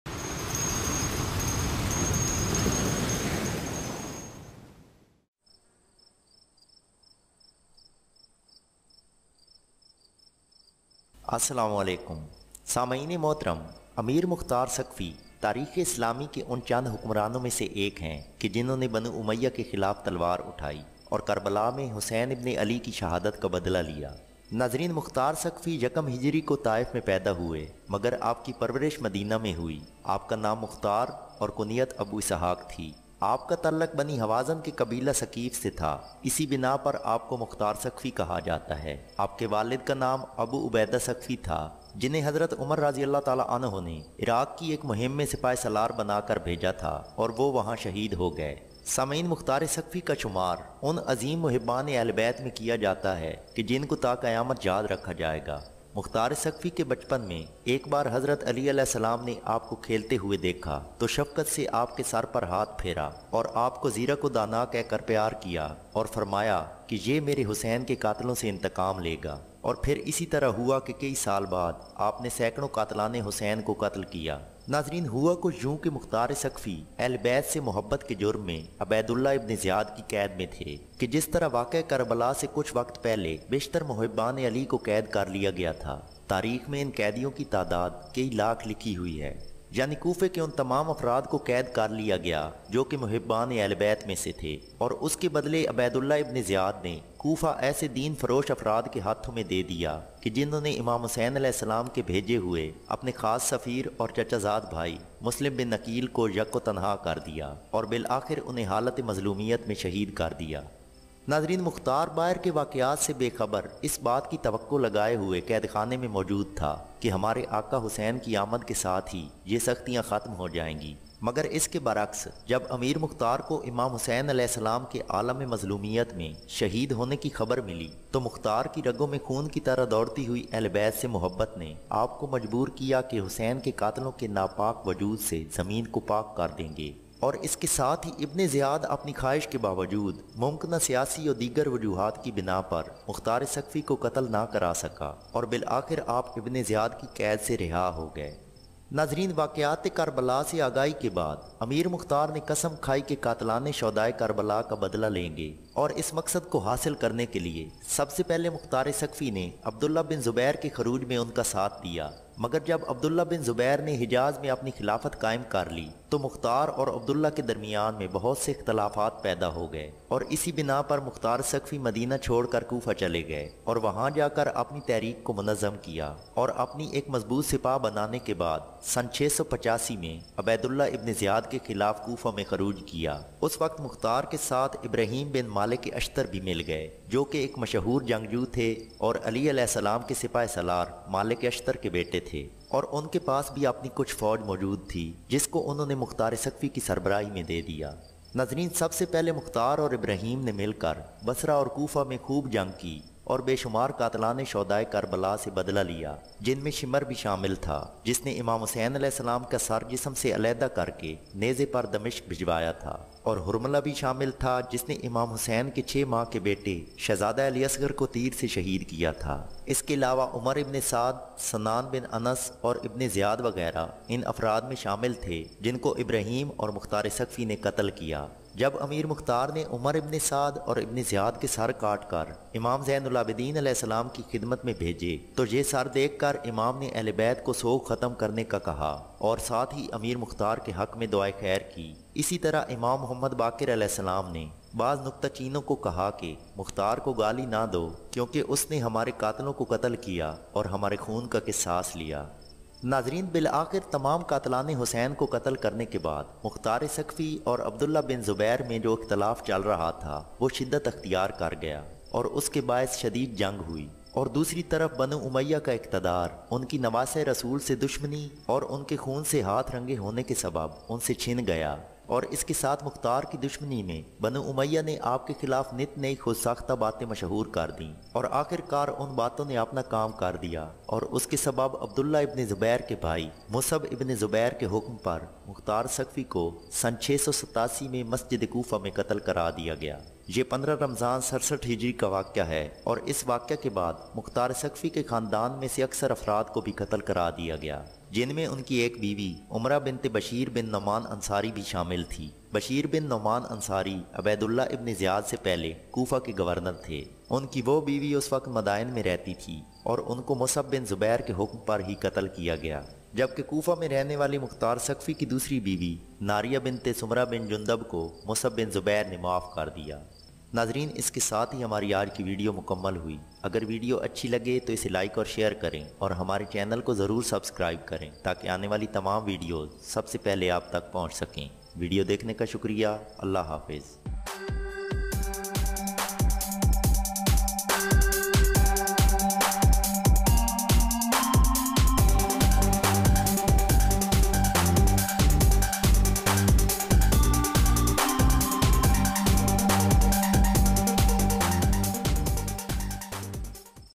Life Story Of Mukhtar al-Thaqafi | امیرمختارثقفی | Documentary In Hindi/Urdu
is video men hum ap ko Amir Mukhtar Saqafi men k bary men chand munfrid malumaat frham kren gy. umeed krty hen k apko hmari ye koshish zarur pasand ay gi or ap k ilm men yaqini izafy ka sbab bny gi.